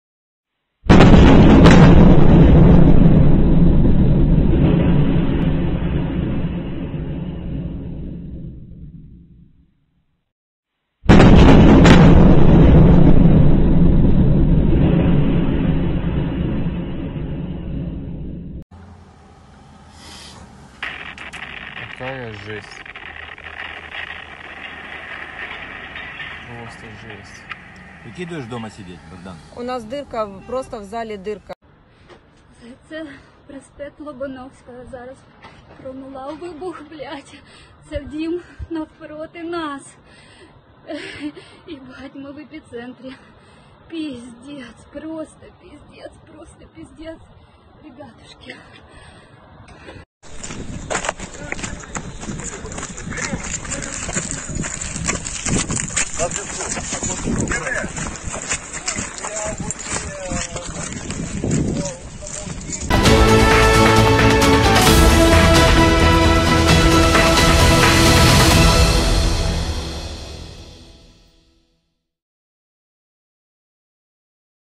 KİEV'DE SİVİLLERİN YAŞADIĞI BİNANIN VURULMA ANI
Rusya'nın Ukrayna'ya saldırısında Rus askeri birlikleri Kiev'e girmeye çalışmaları devam ederken sivillerinde olduğu bazı binalara ateş açıldı.
Kiev'de isabet alan bir dairenin içinde bulunan güvenlik kamerasına o anlar yansıdı. Füze ile vurulan bina tahliye ediliyor.